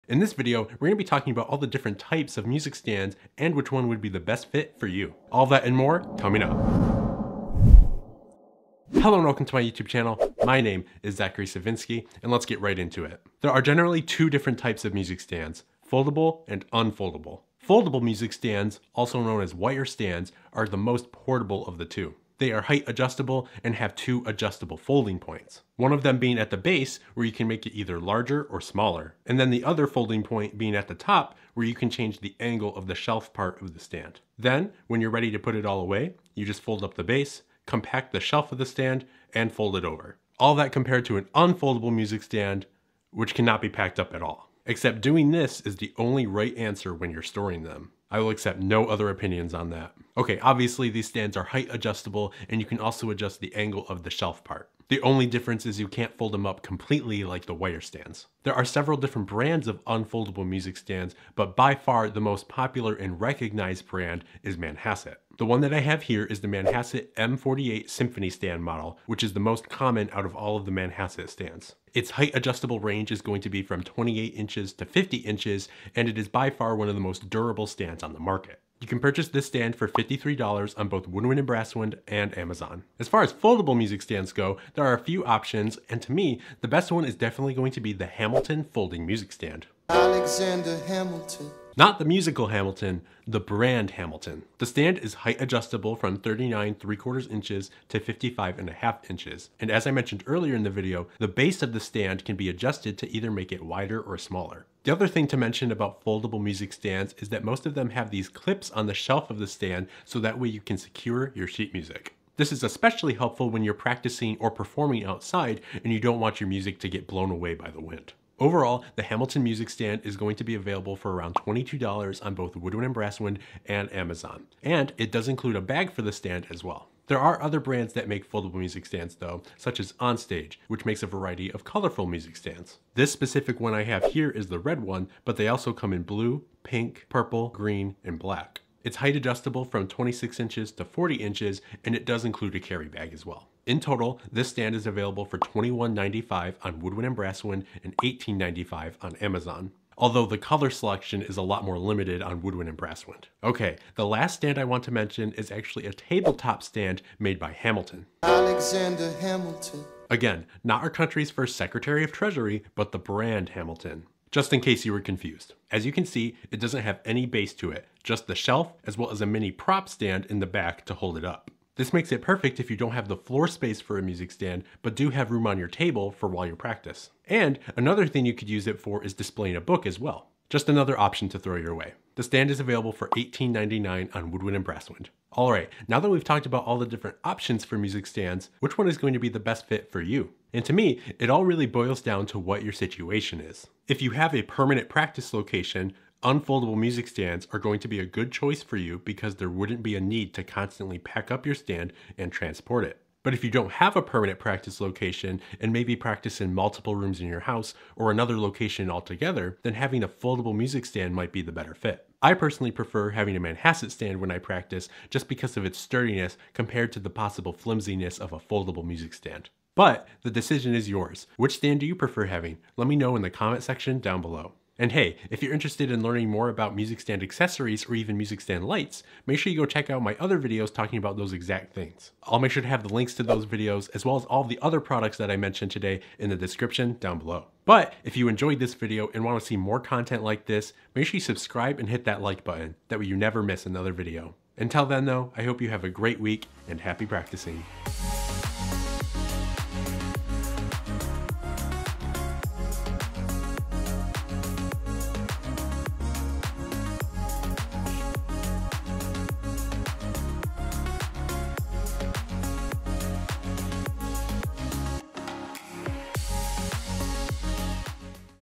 Best Music Stands | Review & Comparison
MUSIC STANDS are one of the MOST essential things we need as musicians… outside of our instrument and the music. But which music stand is the best? FOLDABLE or UNFOLDABLE? All of these questions and more are answered in this video!

MENTIONED IN THIS VIDEO:

Manhasset M48 Symphony Stand

Hamilton KB400N Folding Stand

Hamilton KB70 Tabletop Stand

On-Stage SM7122 Folding Stand